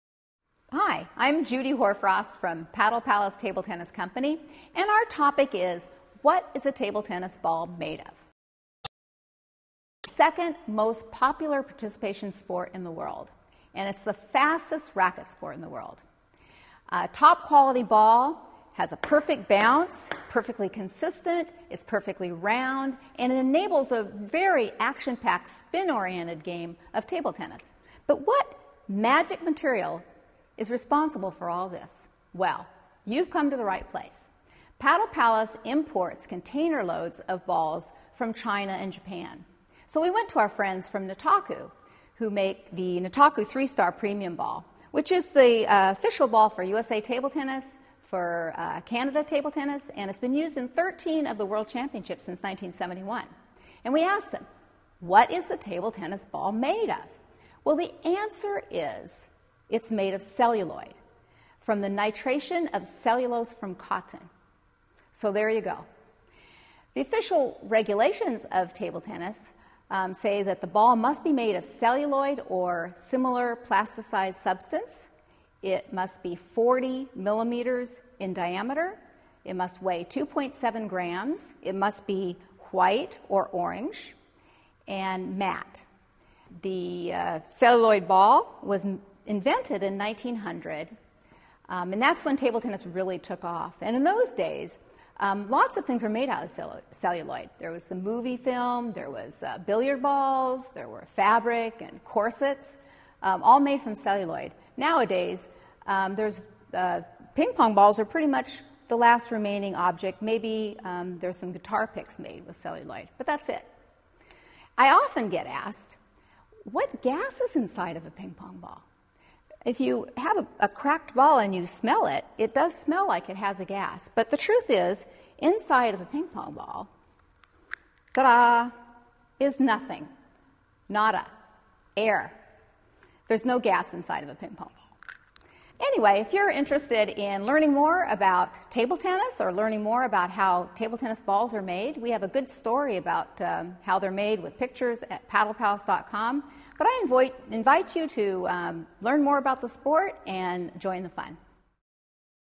What Is a Ping-Pong Ball Made of?
What Is a Ping-Pong Ball Made of?. Part of the series: Ping-Pong Tips. A ping-pong ball is typically made of celluloid from the nitration of cellulose from cotton. Discover how regulations require ping-pong balls to be made of celluloid or a similar substance with help from the owner of a table tennis company in this free video on ping-pong balls.